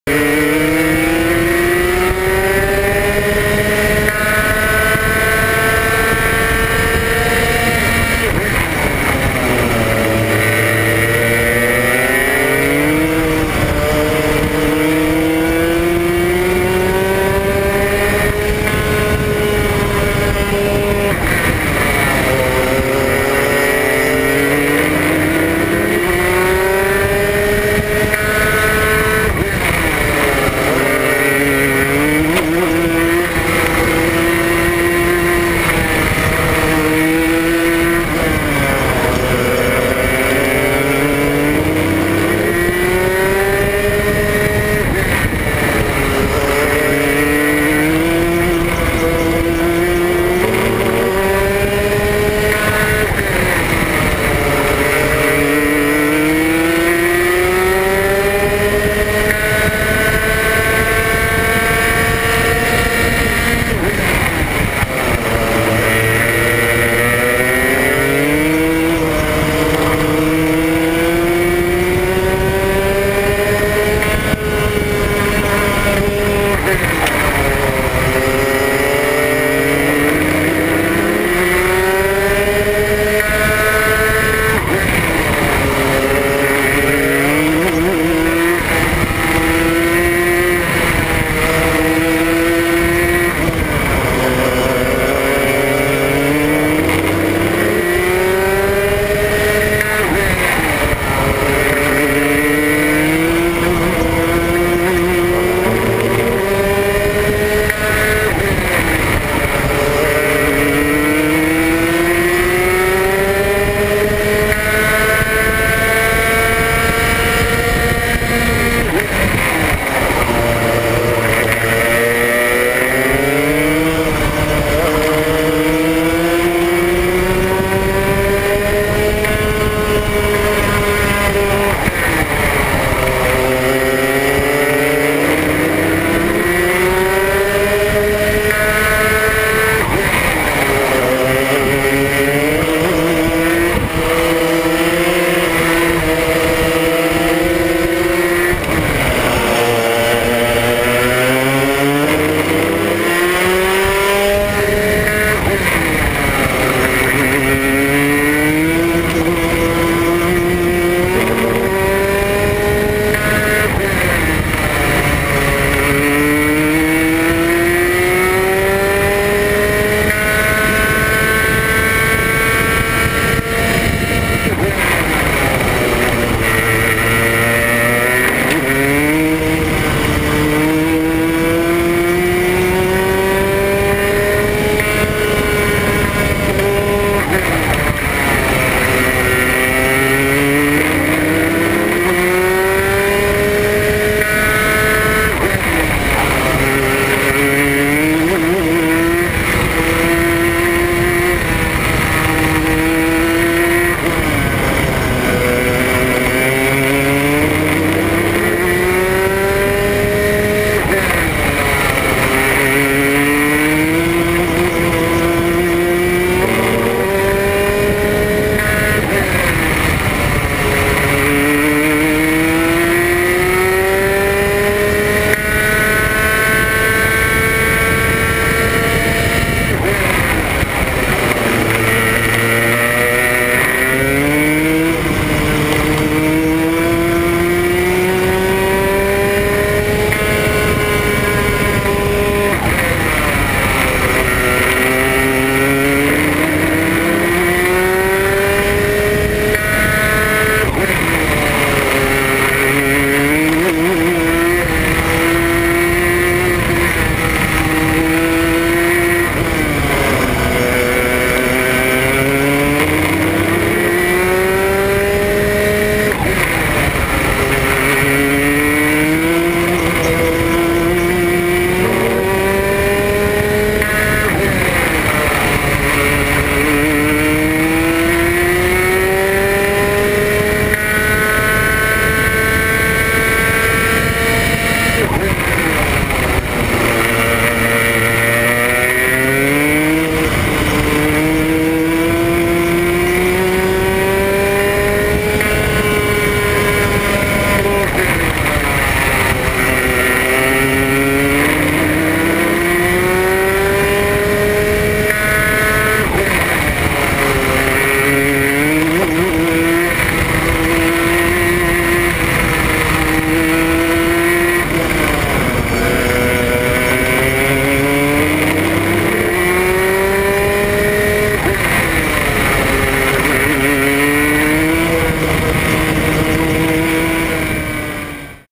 Kenny Vermeylen laps around Genk for the Rotax EuroChallenge
This is how Thursday practice looked like for Kenny Vermeylen onboard his Tecno/Rotax Max at the Rotax EuroChallenge first race in Genk.

CONNECT WITH « THE RACEBOX » ON SOCIAL MEDIA!

Interested in photo coverage, race reports, highlight videos, or even complete branding strategy?
Category